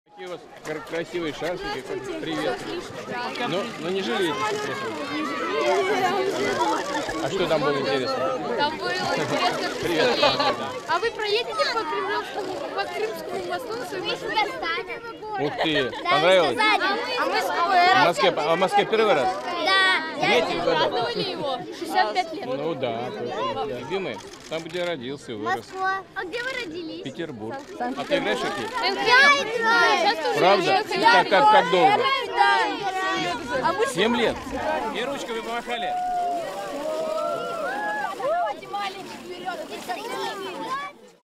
Putin meets children invited to the Kremlin's New Year Party
Russian President Vladimir Putin meets with children on Cathedral Square. Orphans from across the country were among the schoolkids invited. More than 5,000 children received an invitation.